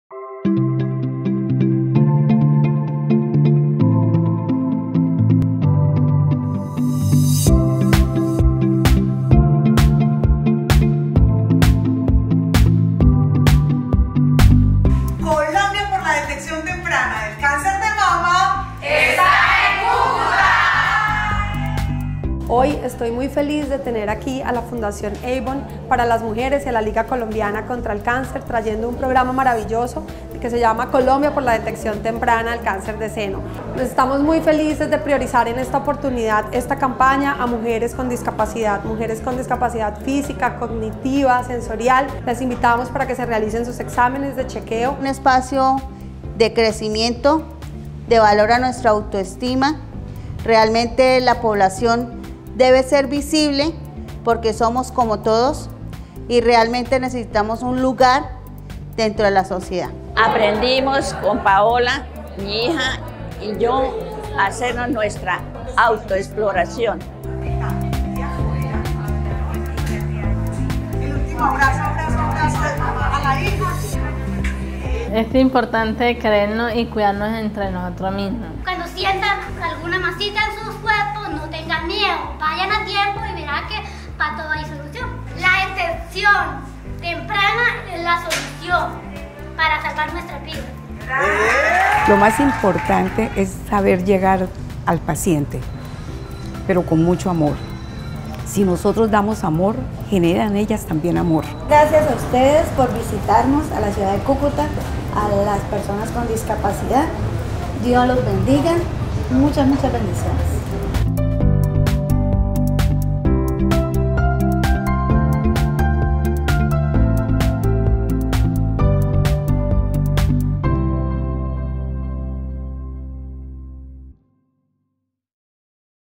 Colombia por la detección temprana del cáncer de mama - Cúcuta
En alianza con Liga Contra el cáncer de Cúcuta priorizamos a las mujeres en condición de discapacidad de Cúcuta, quienes se beneficiaron del programa de detección temprana del cáncer de mama 🤍

¿Qué hicimos?

✋ Autoexploración con 'A' de autoestima
🙎‍♀️ Chequeos clínicos
💜 Mamografías